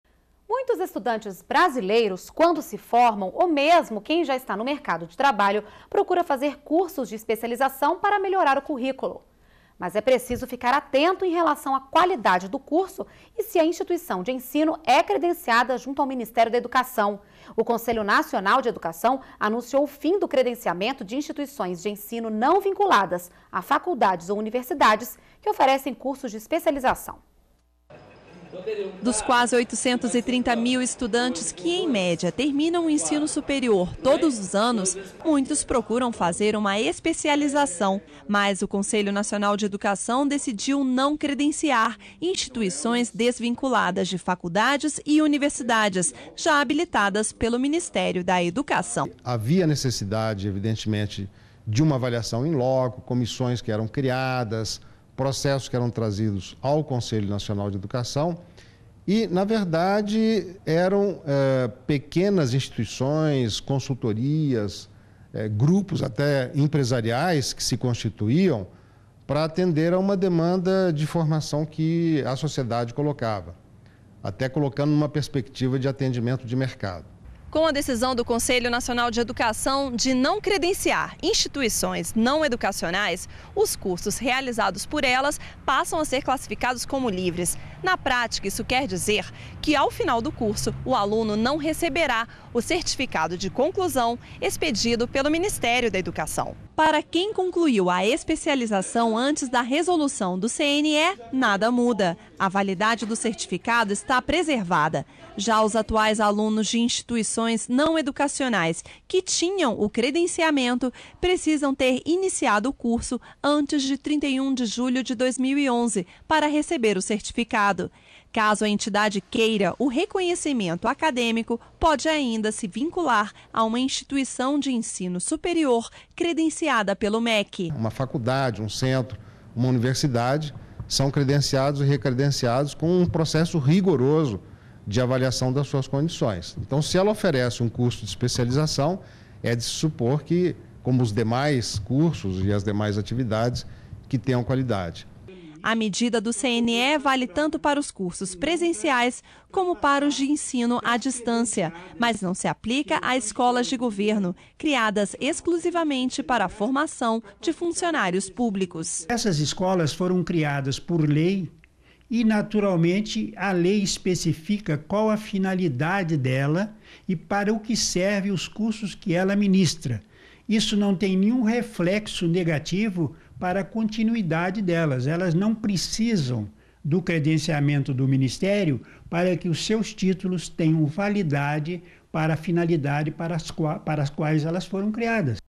MEC muda regras para credenciamento de instituições de ensino
Muitos estudantes brasileiros quando se formam, ou mesmo quem já está no mercado de trabalho, procuram fazer cursos de especialização para melhorar o currículo.
Mas é preciso ficar atento em relação a qualidade do curso e se a instituição de ensino é credenciada junto ao Ministério da Educação. O Conselho Nacional de Educação anunciou o fim do credenciamento de instituições de ensino não vinculadas a faculdades ou universidades que oferecem cursos de especialização.